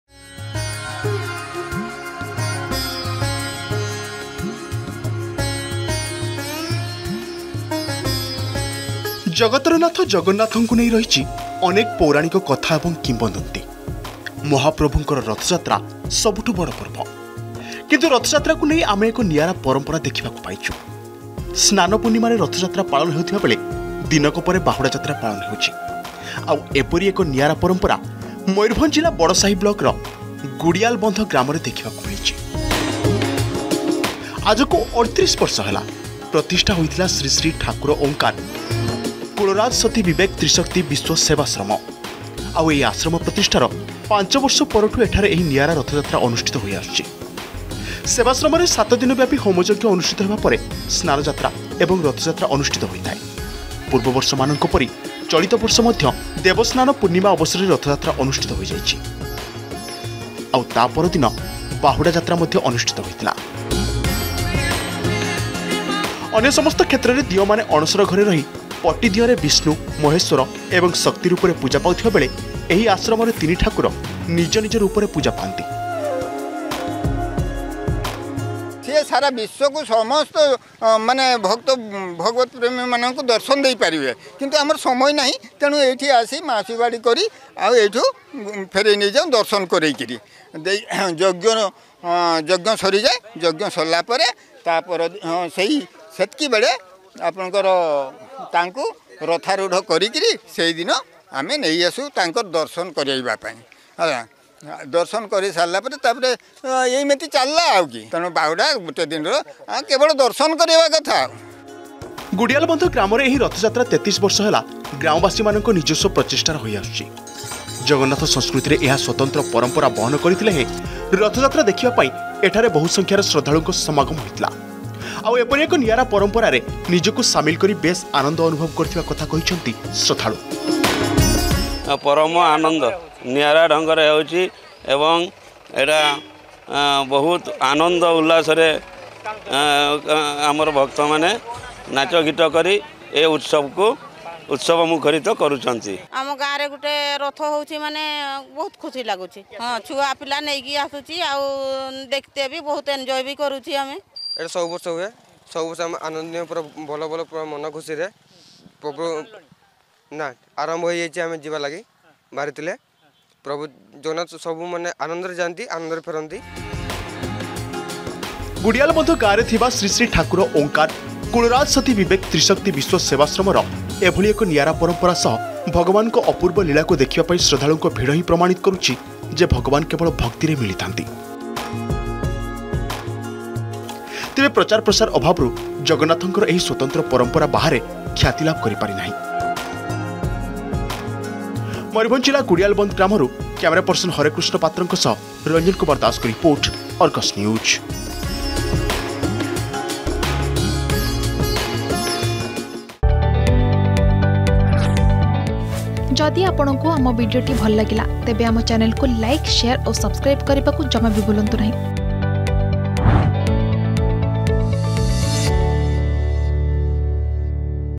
ଜଗନ୍ନାଥଙ୍କୁ ନେଇ ରହିଛି ଅନେକ ପୌରାଣିକ କଥା ଓ କିମ୍ବଦନ୍ତୀ || Ratha Yatra || Mayurbhanj
ନିଆରା ପରମ୍ପରା ମୟୂରଭଞ୍ଜ ଜିଲ୍ଲା ବଡ଼ସାହି ବ୍ଲକର "ଗୁଡିଆଲବନ୍ଧ" ଗ୍ରାମରେ ଦେଖିବାକୁ ମିଳିଛି।  ଆଜକୁ ୩୮ ବର୍ଷ ହେଲା ପ୍ରତିଷ୍ଠା ହୋଇଥିଲା ଶ୍ରୀ ଶ୍ରୀ ଠାକୁର ଓଁକାର କୁଳରାଜ ସତୀ ବିବେକ ତ୍ରିଶକ୍ତି ବିଶ୍ବ ସେବାଶ୍ରମ । ଆଉ ଏହି ଆଶ୍ରମ ପ୍ରତିଷ୍ଠାର ୫ବର୍ଷ ପରଠୁ ଏଠାରେ ଏହି ନିଆରା ରଥଯାତ୍ରା ଅନୁଷ୍ଠିତ ହୋଇ ଆସୁଅଛି ।